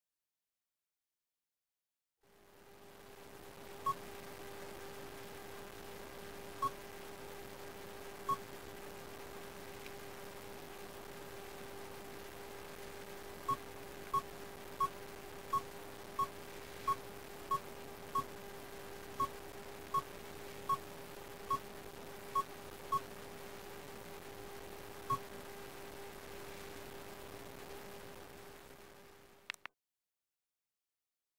Typing on the Nokia 5800 in Full Screen Qwerty mode
The Nokia 5800 runs Symbian S60 V5. We used the stylus so that you could see the screen, you can use your fingers to type too.